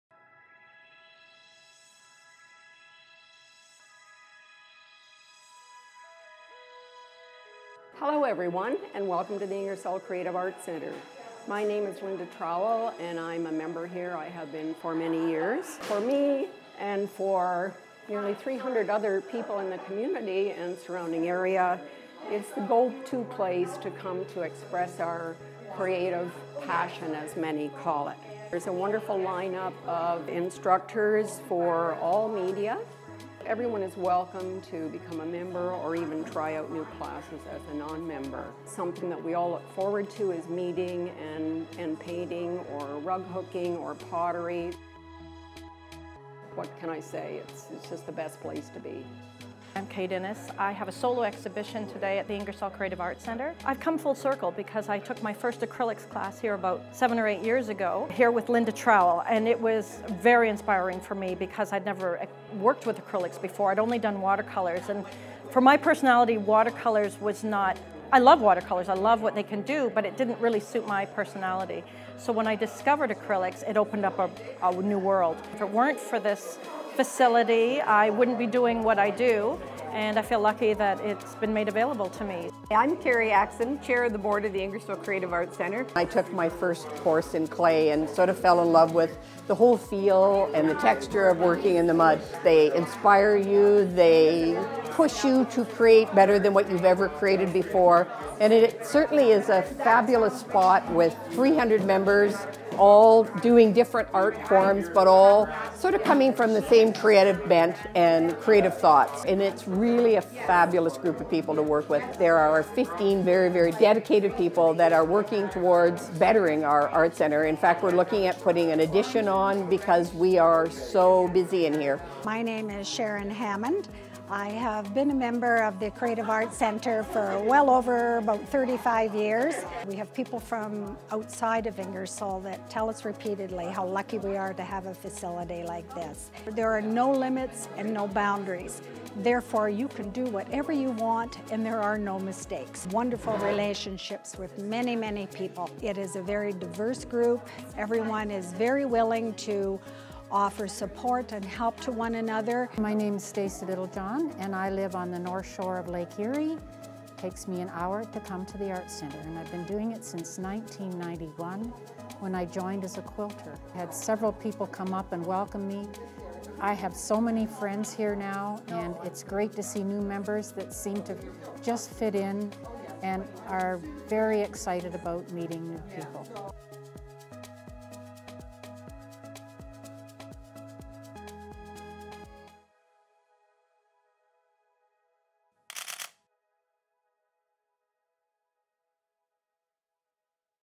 Ingersoll Creative Arts Center
The centre includes memberships and classes in:
Fine Arts, Pottery, Quilters,Rug Hookers, and Fibre Arts
For all ages and levels.
Video created and produced by - Kricket Shots - kricketshots.com